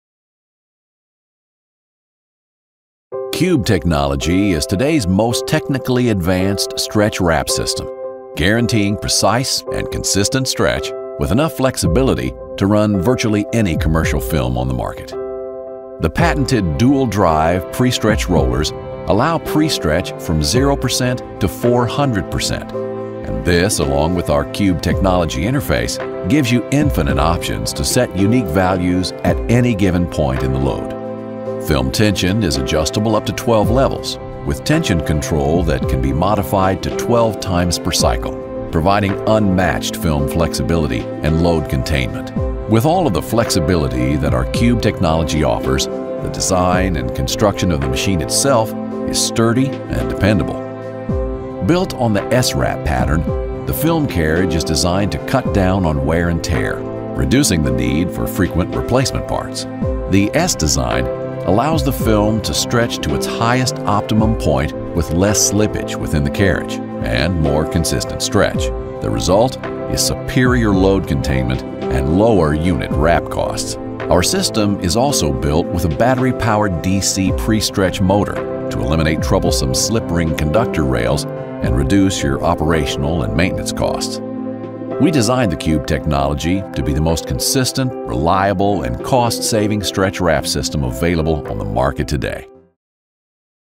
Cube Technology™ by Robopac
Deliver EXACTLY the right amount of film
at EXACTLY the right position
with EXACTLY the right containment force.

- Reduces film cost by 30% to 55%
- Improves load containment
- Reduces product damage by 40%

Cube Technology is:
• Multi-level variable pre-stretch
• Multi-level variable containment force
• Strategic film placement
• ProActive corner compensation

Svolgere ESATTAMENTE la necessaria quantità di film
ESATTAMENTE nella posizione giusta
con ESATTAMENTE la corretta forza di contenimento

- Riduzione dei costi di materiale dal 30% al 55%
- Miglioramento del contenimento del pallet
- Riduzione dei danni al prodotto del 40%

Cube Technology è
• Prestiro variabile multi-livello
• Forza di contenimento variabile multi-livello
• Posizionamento del film strategico
• Sistema di compensazione degli angoli
del pallet “ProActive”